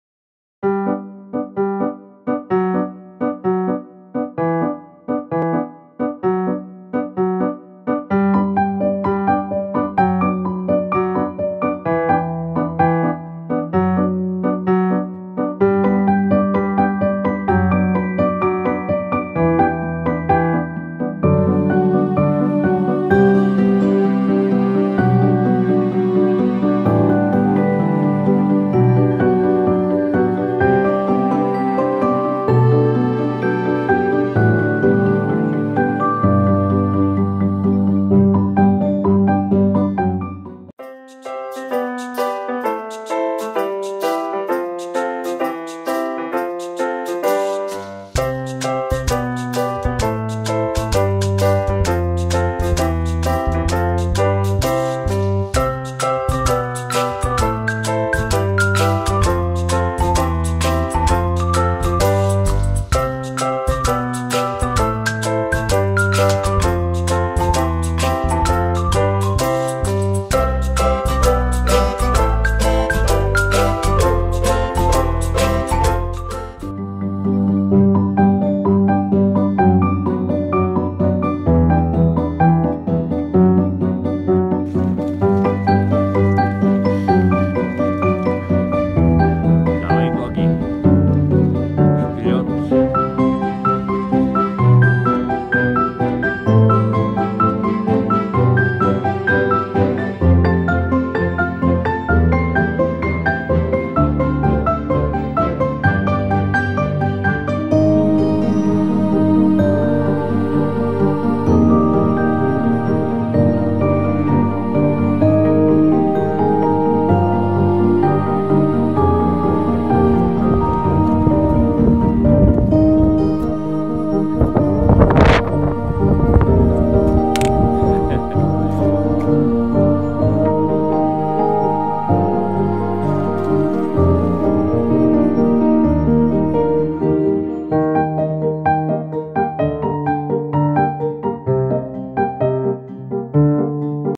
Andrey's Movie about our 2016 winter hike to Musala, Rila Bulgaria
Andrey and Yuri created this movie about the winter climb to Musala (highest peak of the Rila Mountains/ Bulgaria)  - from idea/ planning to execution and documentation  ;)  .. with Cyrillic Bulgarian transitions - you test your language skills :)